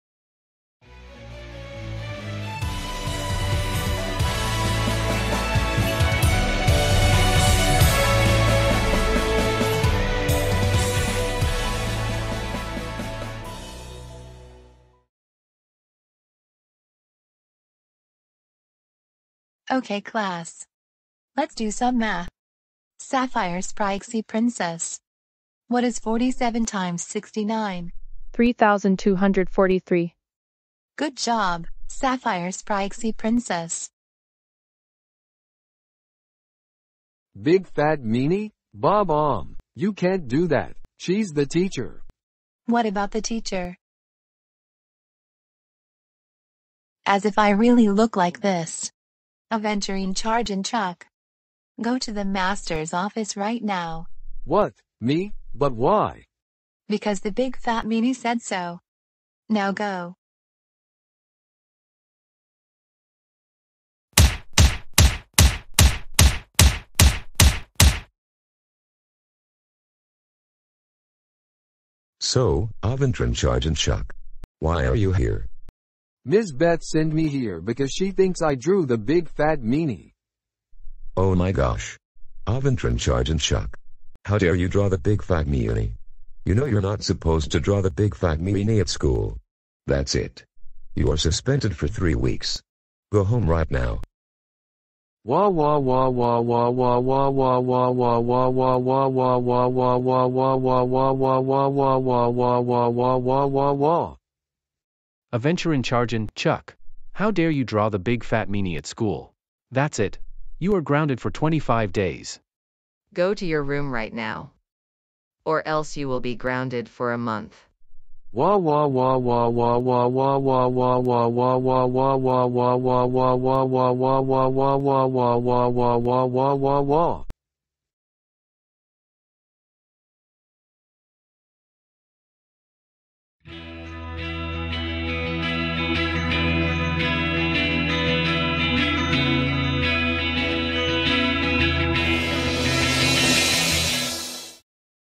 Aventurine Chargin’ Chuck Draws The Big Fat Meanie/Grounded
Cast For This Video:

Aventurine Chargin’ Chuck: en-US-Standard-B
Aventurine Chargin’ Chuck’s Dad: Ryan (Multilingual) (United States)
Aventurine Chargin’ Chuck’s Mom: AlGenerate2 (Neural) (United States)
Sapphire Sprixie Princess: ???
Ms. Beth: Beth (US)
Principal Dave: Dave (US)

Note: I’m Not Gonna Make This Types Of Videos And Move On From Now! I’ll Find School Videos That Are Shorter Or Long!